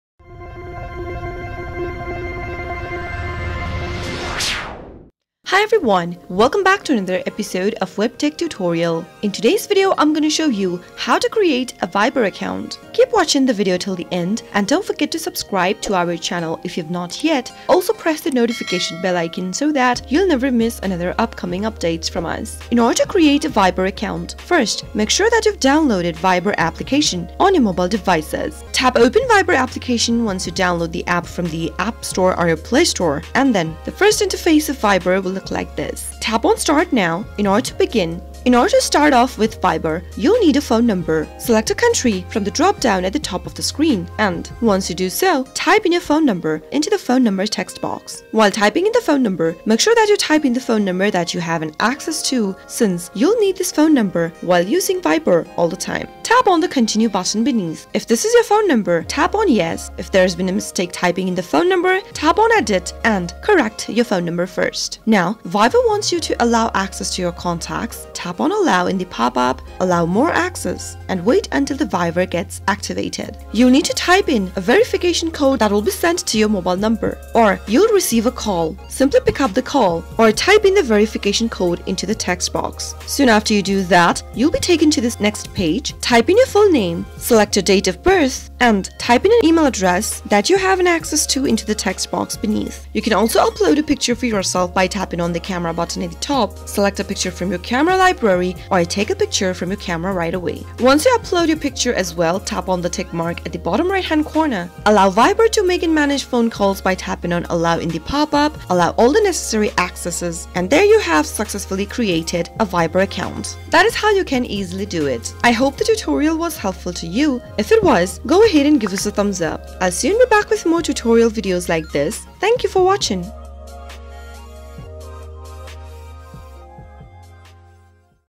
Viber Sign Up: How to Create/Open Viber Account from Android Mobile? (2022)
See step by step tutorial on creating an account on Viber.

CHAPTERS

0:22 - Create Viber Account | Viber Account Sign Up
1:55 - Outro